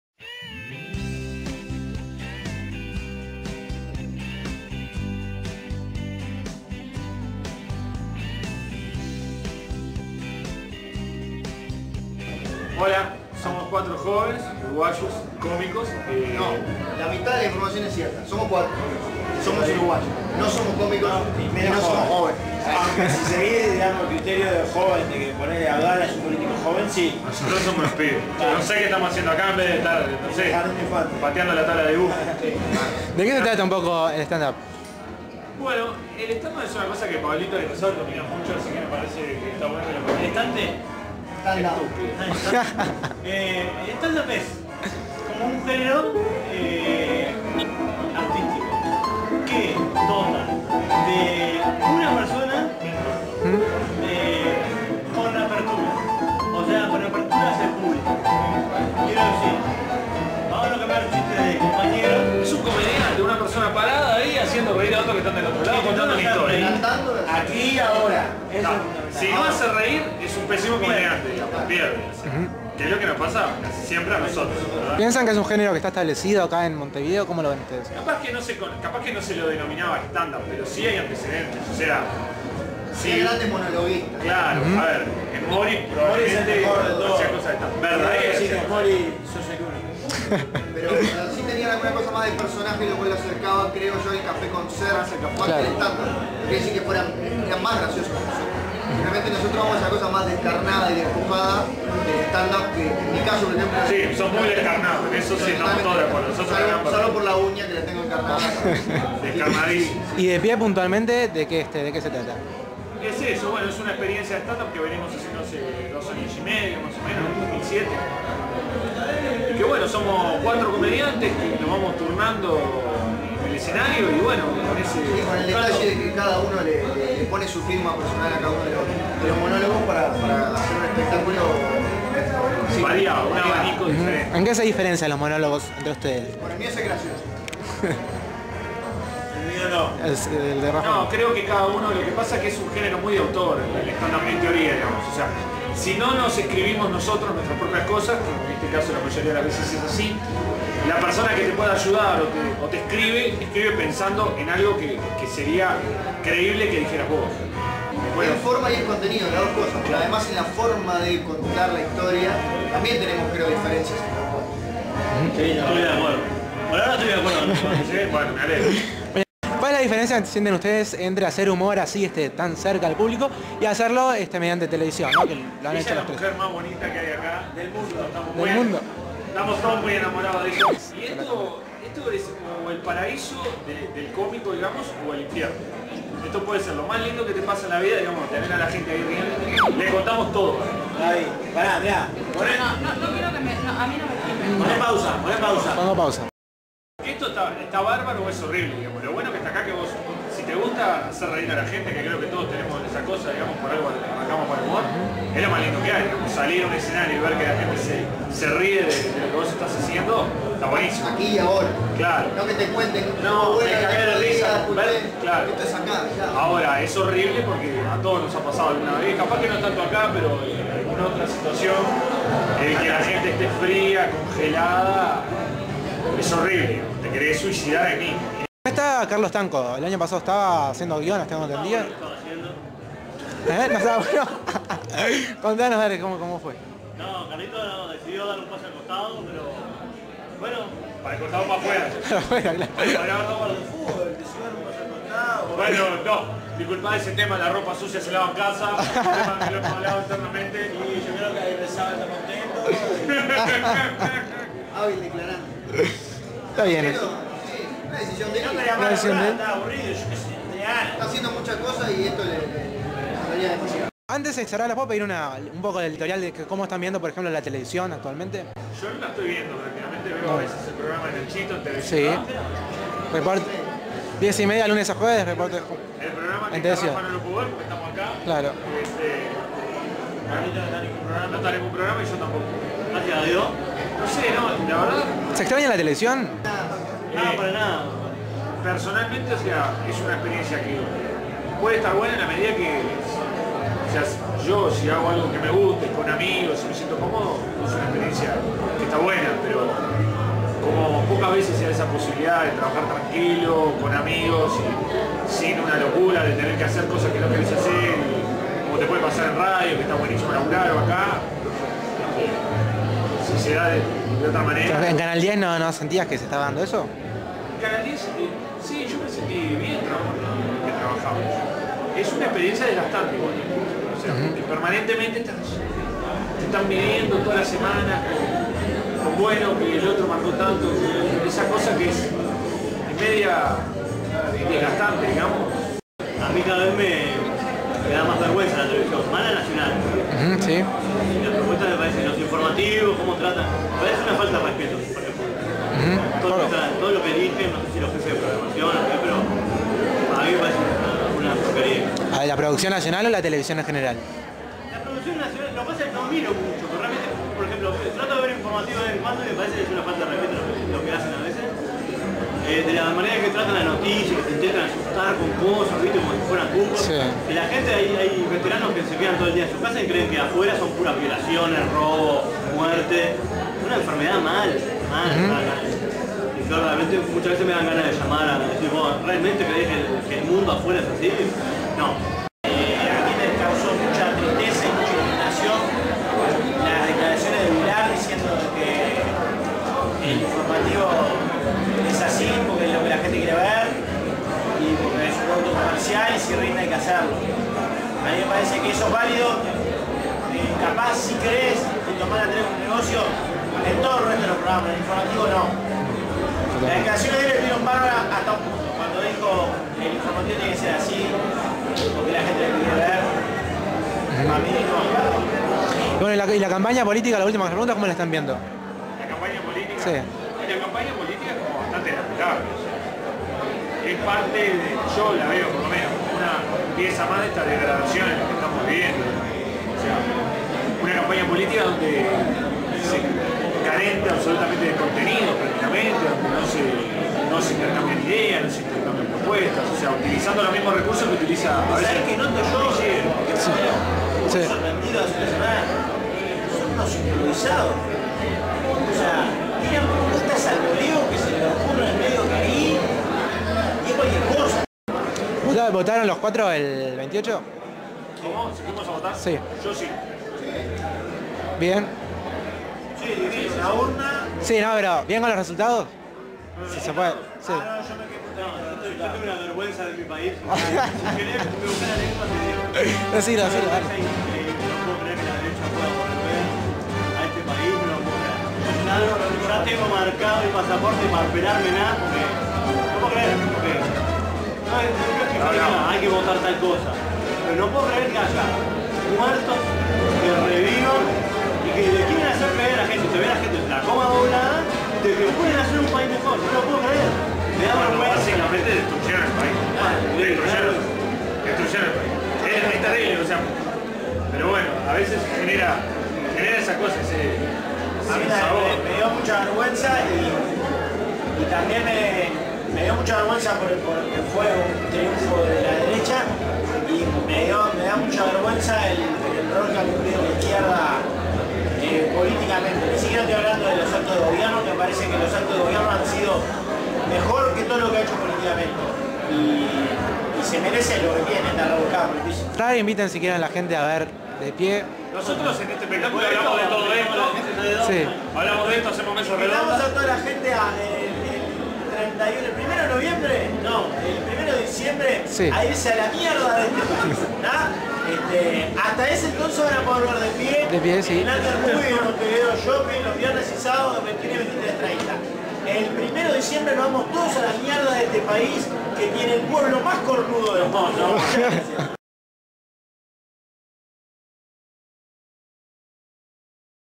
CÁMARA TELEMEDIOS: "DE PIE 2"
Cámara Telemedios con el elenco de "De Pie 2": Ignacio Alcuri, Rafa Cotelo, Pablo Aguirrezábal y Gonzalo Cammarota.